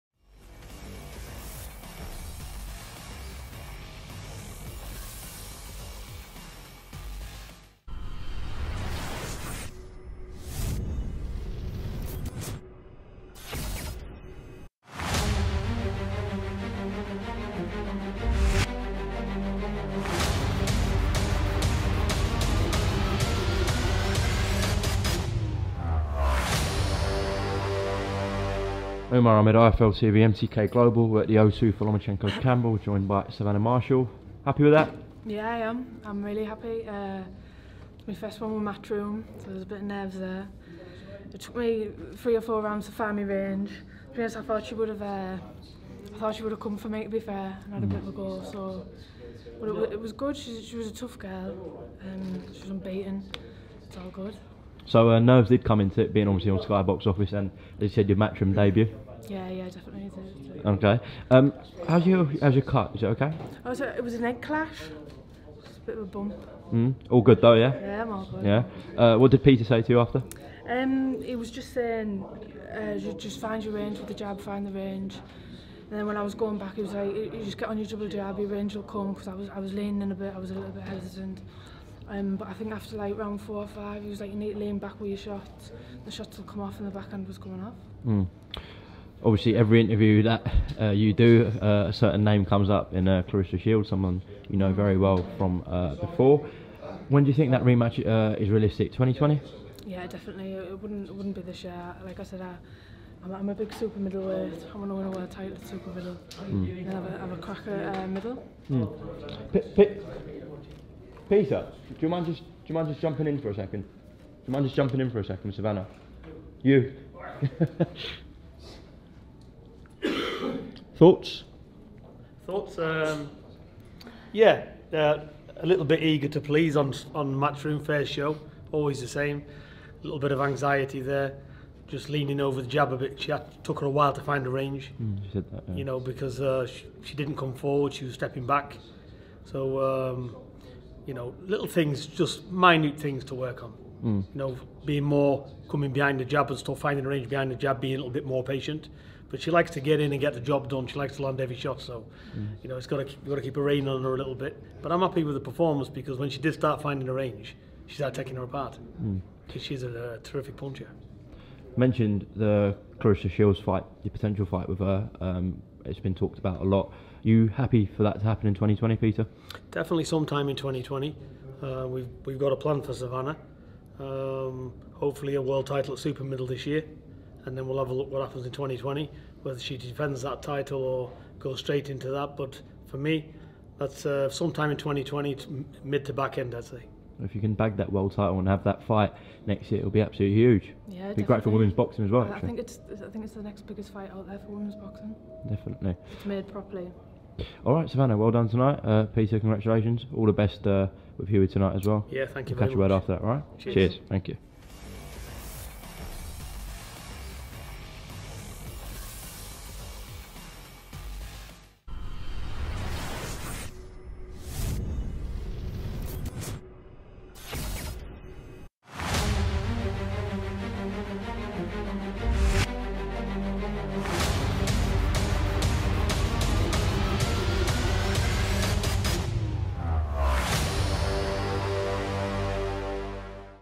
'THE CLARESSA SHIELDS FIGHT WILL DEFINITELY HAPPEN IN 2020!' - PETER FURY & SAVANNAH MARSHALL
*POST-FIGHT INTERVIEW BY UMAR AHMED*